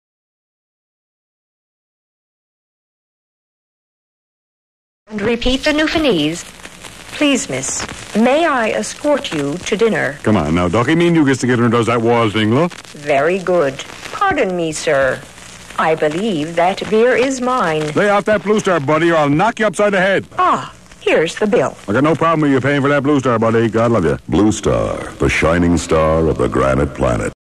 Early 1990's Blue Star Beer Commercial from Newfoundland Canada
I taped this when I was in college. I loved this commercial. Love the beer also.